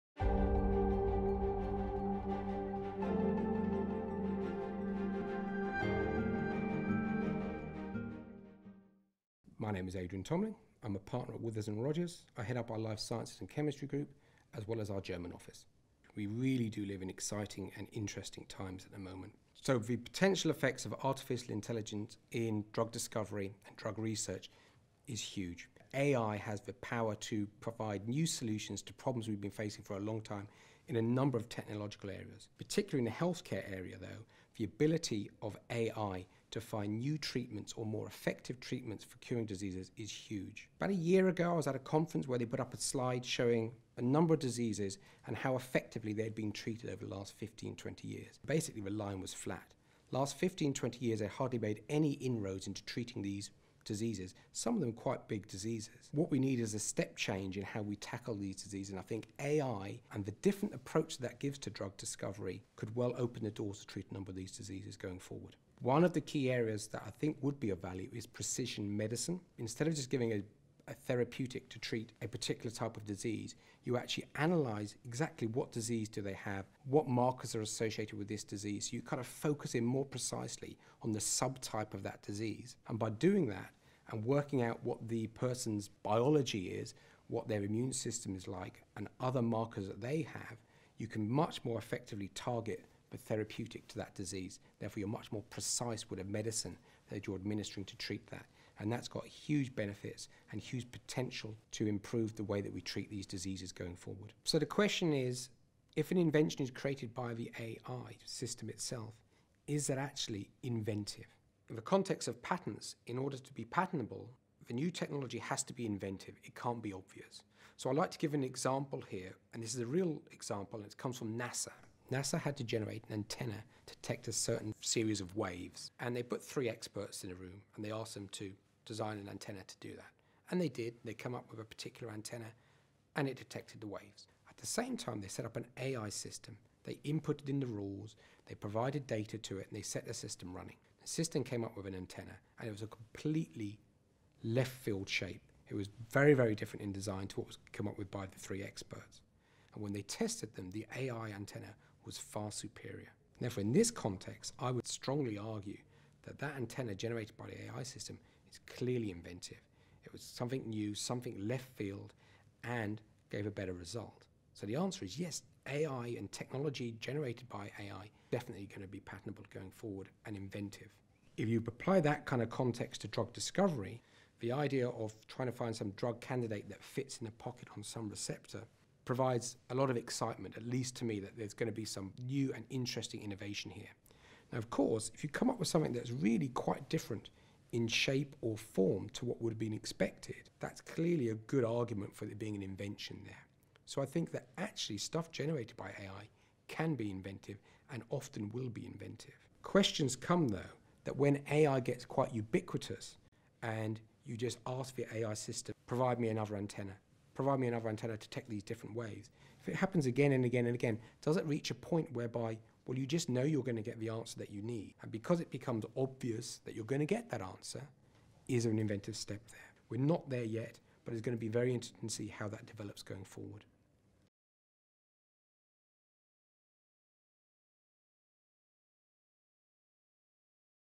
Innovation by Artificial Intelligence and the role of AI in Pharmaceutical discovery
Withers & Rogers Partner, Adrian Tombling discusses the potential effects of artificial intelligence in the pharmaceutical sector.